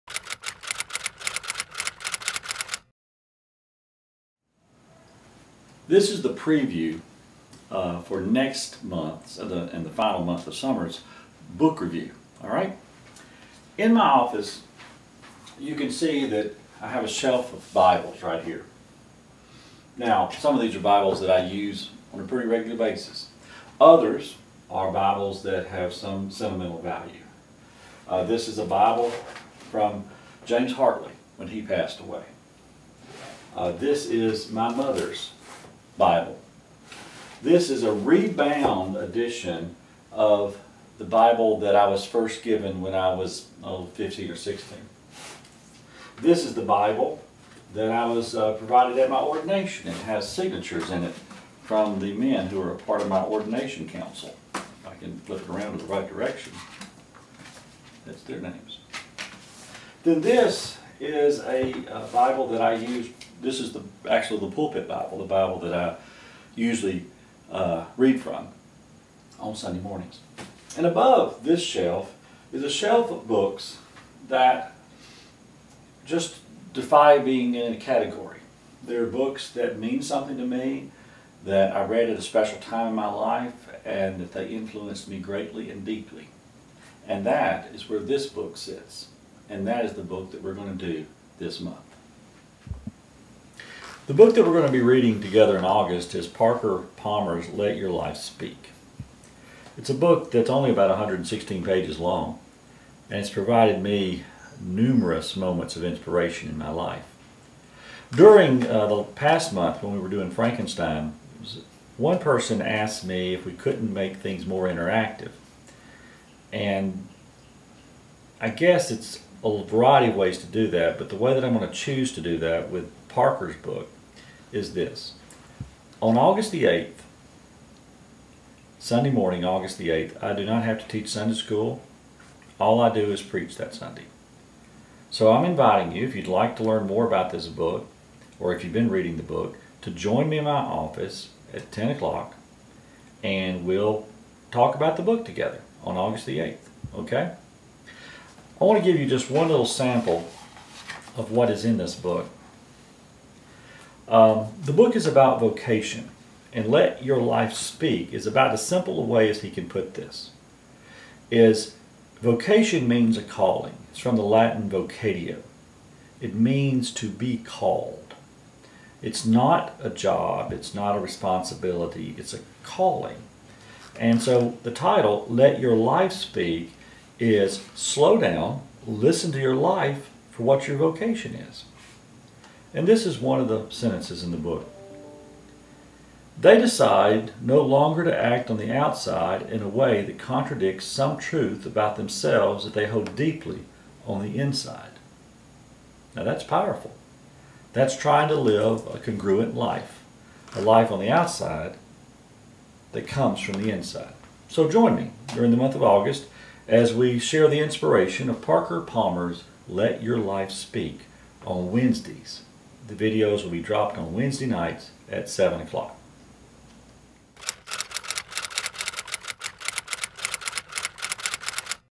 August Book Club Intro * Let Your Life Speak
With this video, John invites you into an experience as we read "Let Your Life Speak" by Parker Palmer.  Whether you are a fan of Parker Palmer, or have never heard of him, these conversations with John will introduce you to listening to yourself in your life's journey.

From the book cover.. "With wisdom, compassion, and gentle humor, Parker Palmer invites us to listen to the inner teacher and follow its leadings toward a sense of meaning and purpose.  He shares insights gained from darkness and depression as well as fulfillment and joy, illuminating a pathway toward vocation for all who seek the tru calling of their lives"

Beginning August 4, Book Club videos will be available on all social media venues by 7 pm on Wednesdays.